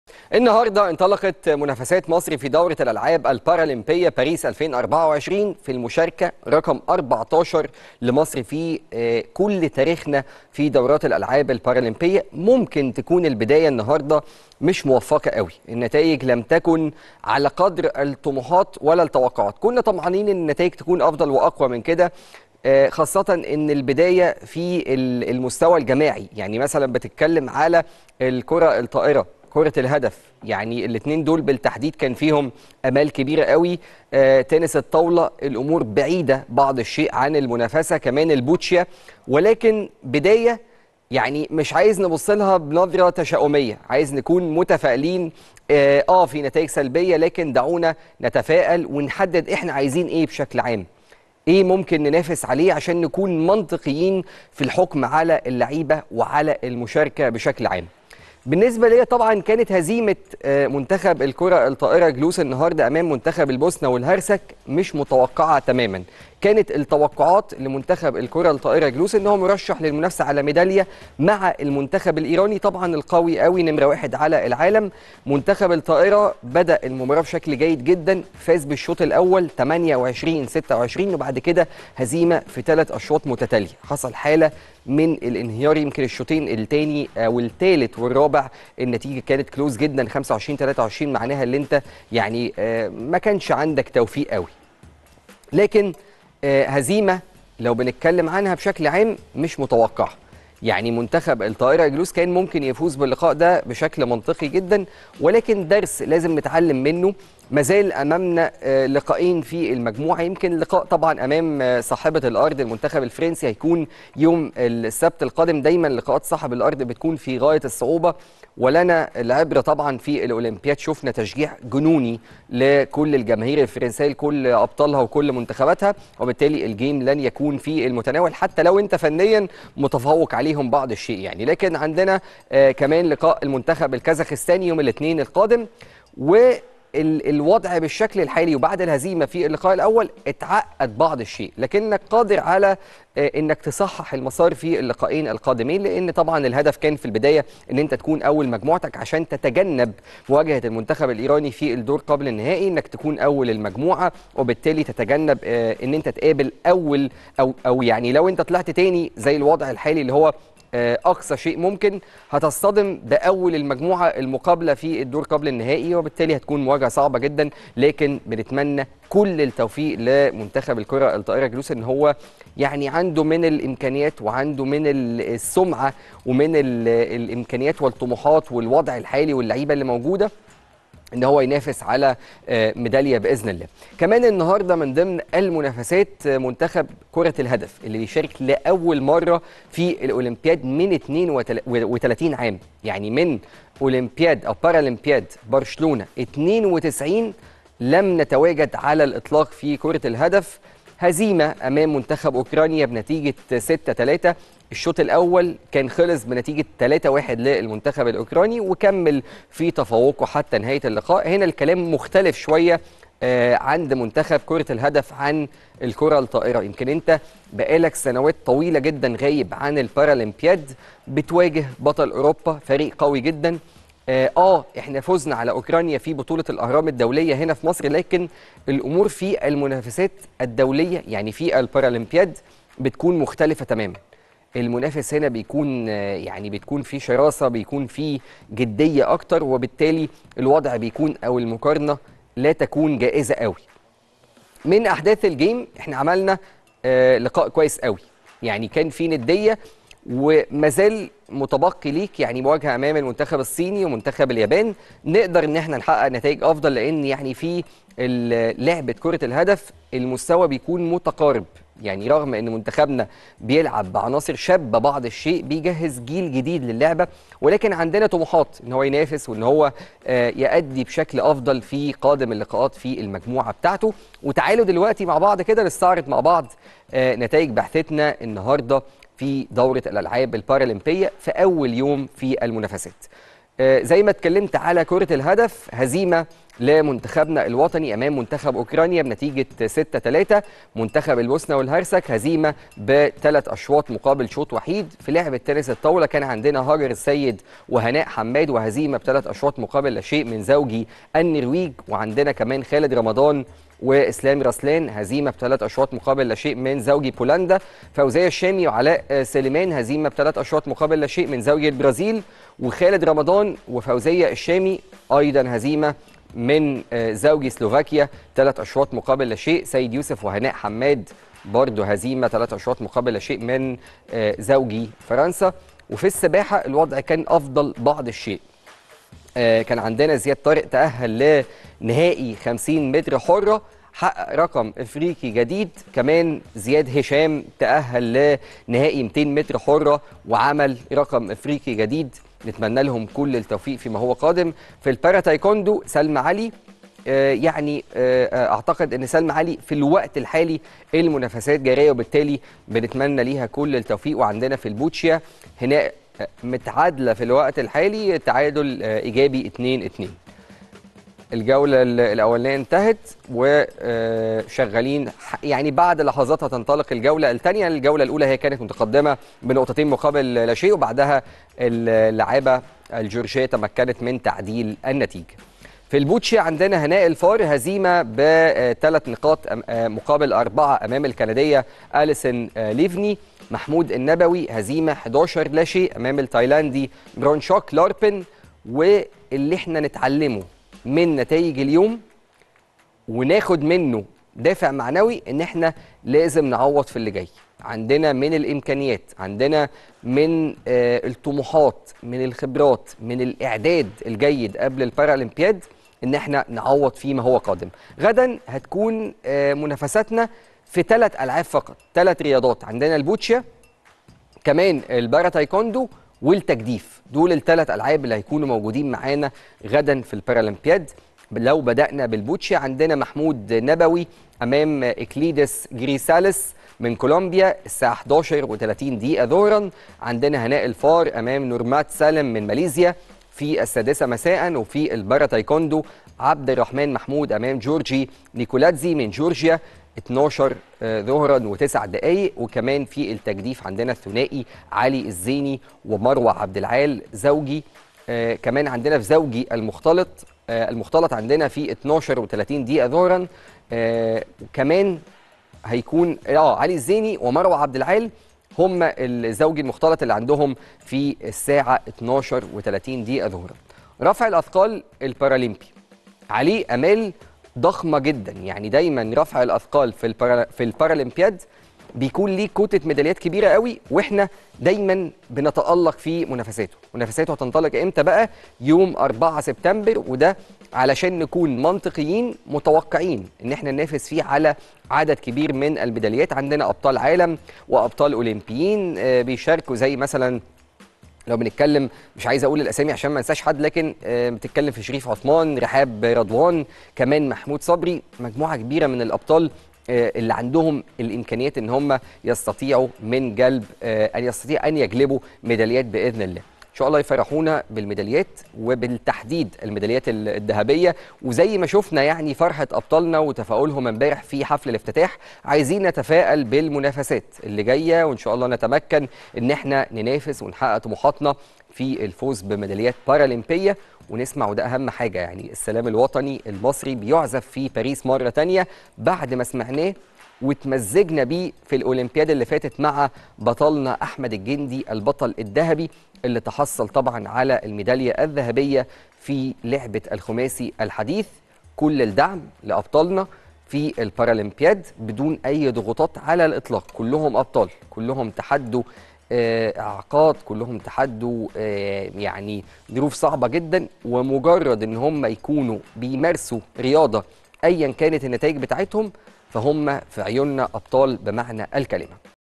أيمن مصطفى يستعرض نتائج بعثة منتخب مصر يوم الخميس 29 أغسطس في بارالمبياد باريس 2024
ملاعب الأبطال - أيمن مصطفى يستعرض نتائح منافسات بعثة منتخب مصر يوم الخميس 29 أغسطس في بارالمبياد باريس 2024